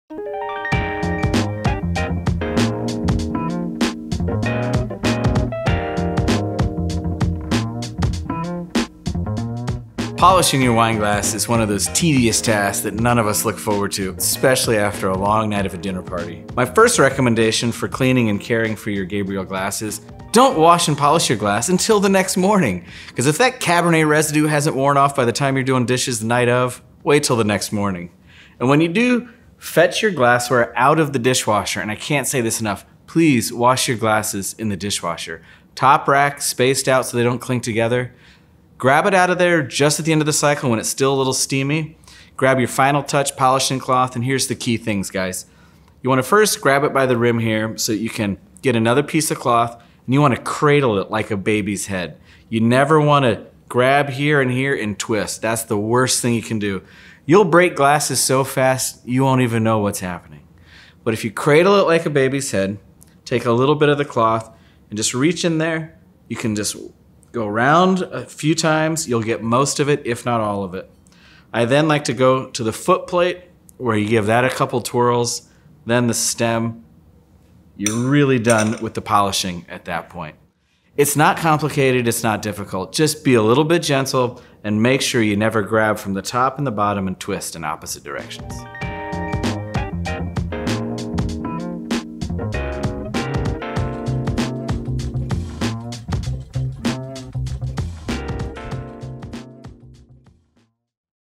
How to Polish Your Gabriel-Glas Crystal Wine Glasses
Listen as Jonah Beer talks about safely and properly polishing a wine glass. There are a few short and quick steps to always have a sparkling wine glass.

-Wash your glasses in the dishwasher
-Polish right after the load finishes
-Cradle the glass and give it a couple of twirls
-Never twist the glass in opposite directions

Cheers! Happy polishing.